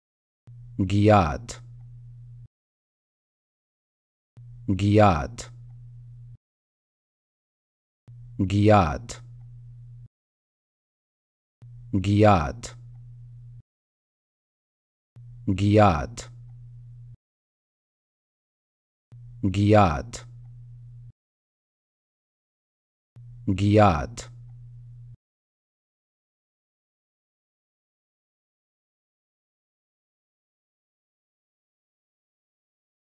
How To Pronounce Gyat
Welcome to our pronunciation guide!
In this video, we'll explore how to pronounce Gyat. If you're seeking guidance on how to say Gyat, this tutorial has you covered.

Cómo Pronunciar Gyat
Como Pronunciar Gyat
Comment Prononcer Gyat
Wie Man Gyat Ausspricht
Как Произнести Gyat
كيف تنطق Gyat